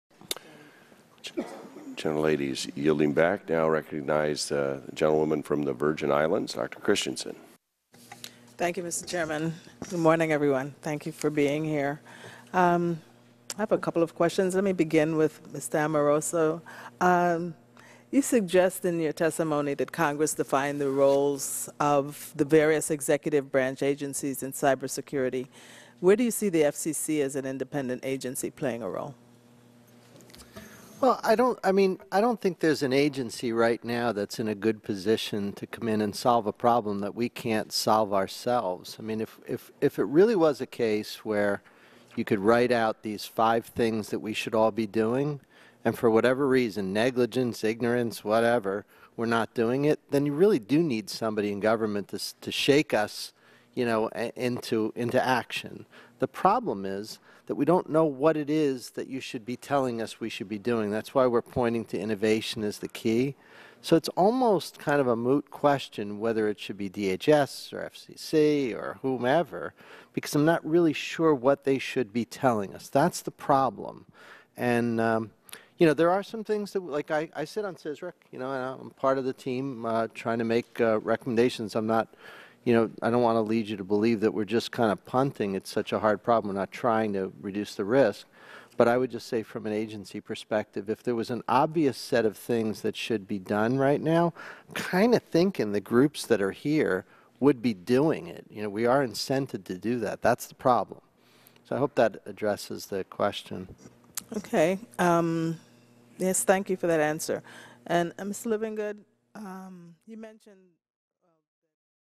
Test PAC - AT&T Vice President: We Can Solve Cybersecurity Ourselves
Companies like AT&T support CISPA, but how far are they willing to go?

As congress draws closer to weighing in on the Cyber Intelligence Sharing and Protection Act (CISPA) later this month, it is becoming increasingly apparent that the fight for sensible internet privacy legislation is not a corporate interest.

Officially backed by twenty-eight companies including Facebook, AT&T, and Microsoft, CISPA aims to fundamentally change the relationship between the private and public sector by encouraging companies to share your personal information with government agencies without penalty.

With the broad support of corporations - the largest of which opposed the Stop Online Piracy Act - it would seem that there would be conclusive incentives for companies welcoming the surrendering of private user information for cyber security means. After all, as Tim McKone, AT&T's Executive Vice President of Federal Relations stated in his letter of support for CISPA:

    "The sharing of cyber threat and attack information is an essential component of an effective cyber-defense strategy, and the legislation helps to provide greater clarity for private sector entities. We commend the bill's sponsors for their leadership on this critical issue."

  While McKone paints a pleasant picture of the interaction between the public and the private sector, AT&T's testimony before the House Energy and Commerce Subcommittee on Communications and Technology shows a company confused and reluctant of government regulation.

When asked by Virgin Islands delegate Donna Christensen about the role of the FCC in combating cyber crime, for instance, AT&T's Senior Vice President and Chief Security Officer Edward Amoroso did not hesitate to outline the inability of the government to have any meaningful solutions:

    "I don't think there's an agency right now that's in a good position to come in and solve a problem that we can't solve ourselves...if there was an obvious set of things that should be done right now, I'm kind of thinking the groups that are here would be doing it. We are incented to do that."

Amoroso's testimony demonstrates that complexity of dealing with cyber security while also admitting that overreaching security laws would do little to resolve the problems at hand. "The problem is," Amoroso explains, "that we don't know what it is that you should be telling us we should be doing."

Additionally, it is important to note that there are already laws in place which allow law enforcement and private companies to share information through subpeonas and other legal means. (Click here for an example of Facebook's compliance with disclosure of information.)

CISPA ultimately attempts  to take the liability away from companies who currently have the discretion of disclosing your information and places the decision solely in the hands of the government; all at the expense of the user.

CISPA would effectively take the door off the hinge of every household in America, but lacks the tools necessary to distinguish whether there is a criminal hiding in the attic. Why surrender the core of our privacy for the sake of corporate and governmental convenience?